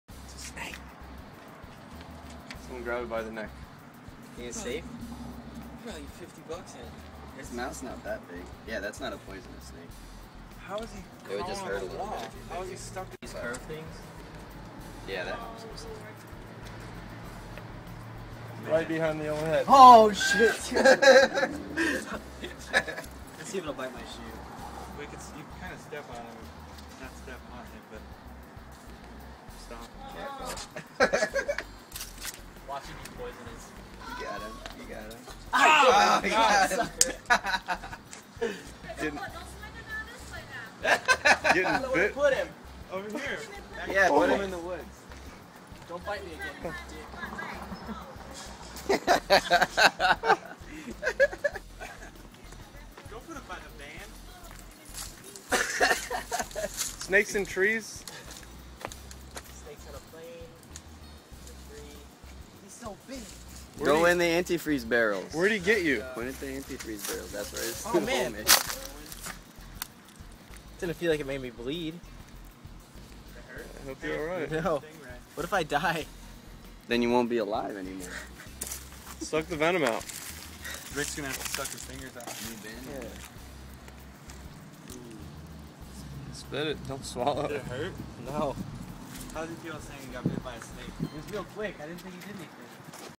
Scene kid bit by snake
DO NOT WATCH THIS VIDEO. Super sexy man Balz from Motionless in White tried to be a hero and wrestle a deadly snake for no apparent reason. Let's see him suck out the venom after he's dead! I'm sure he can feel that one entering his veins. Voodoo 4 lyfe